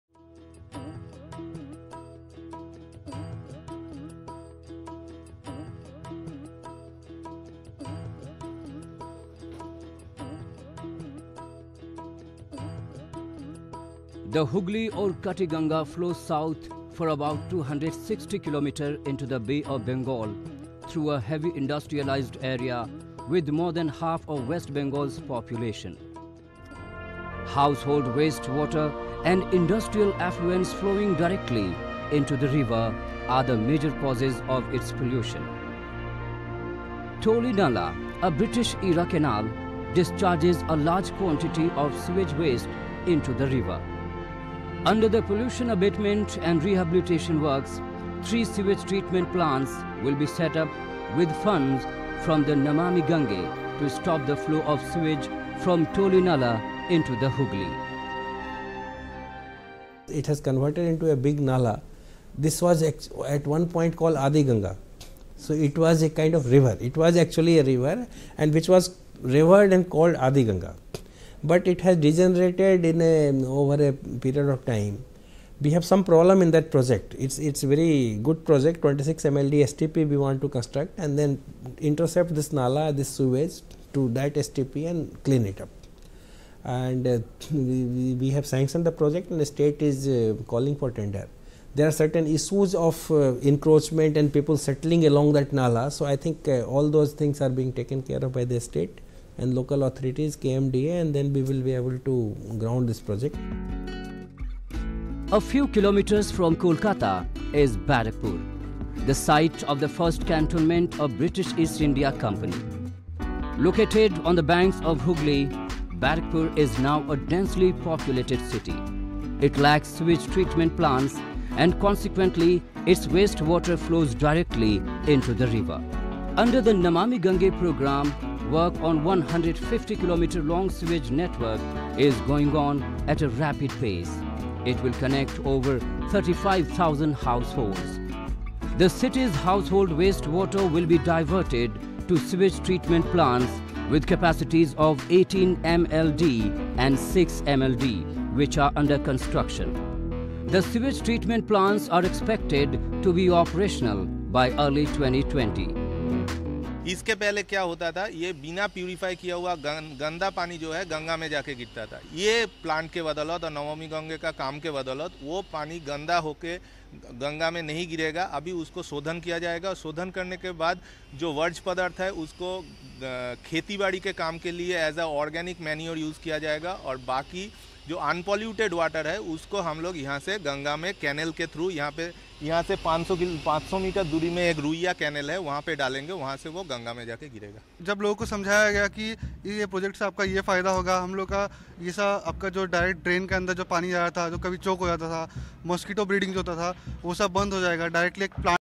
West Bengal: Pollution abatement projects to prevent sewage flow into Ganga
The Hooghly or Kati-Ganga flows south for about 260 km into the Bay of Bengal, through a heavy industrialised area with more than half of West Bengal's population.